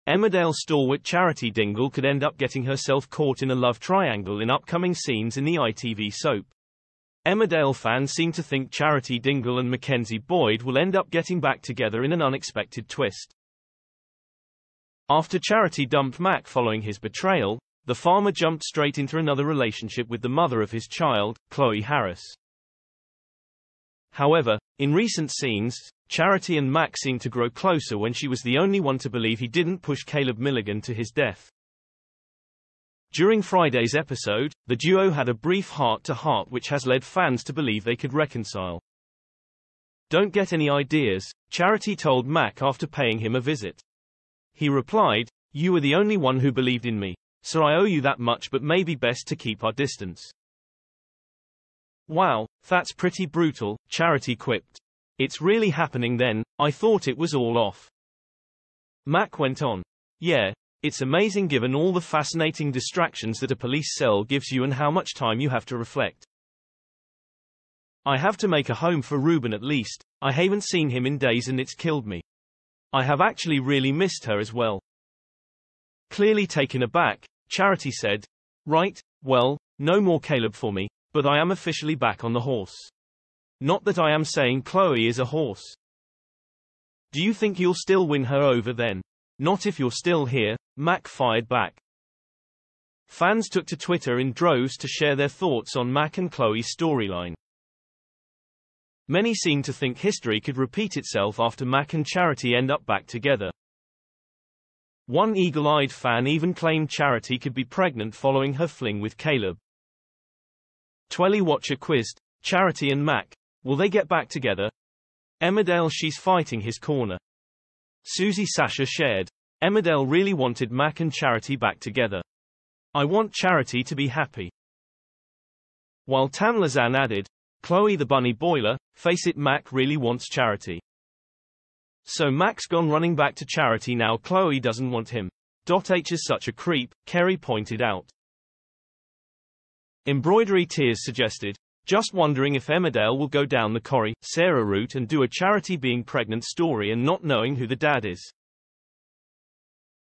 Emmerdale R0CKED by pregnancy as fans 'W0RK 0UT' Mackenzie Boyd cheats again
Emmerdale stalwart Charity Dingle could end up getting herself caught in a love triangle in upcoming scenes in the ITV soap.